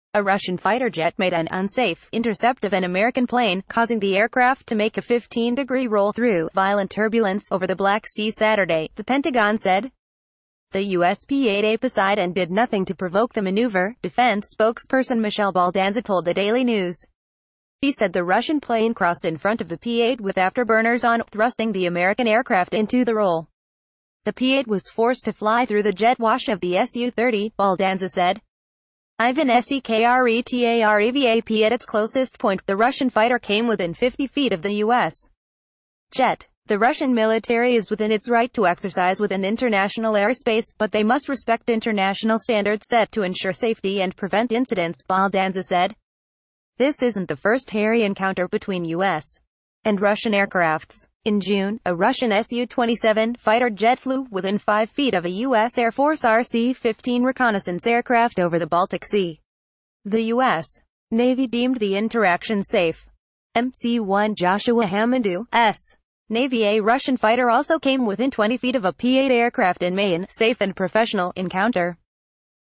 TOP NEWS - Russian fighters blocked the U.S. Navy flew ' unsafe ' interactions on the Black Sea
TOP NEWS HEADLINES DAILY - TOP NEWS - Russian fighters blocked the U.S. Navy flew  unsafe  interactions on the Black Sea

A Russian fighter jet made an “unsafe” intercept of an American plane, causing the aircraft to make a 15degree roll through “violent turbulence” over the Black Sea Saturday, the Pentagon said. The U.S. P8A Poseidon did nothing to provoke the maneuver, defense spokesperson Michelle Baldanza told the Daily News. She said the Russian plane crossed in front of the P8 with  afterburners on, thrusting the American aircraft into the roll. “The P8 was forced to fly through the jet wash of the SU30,” Bal...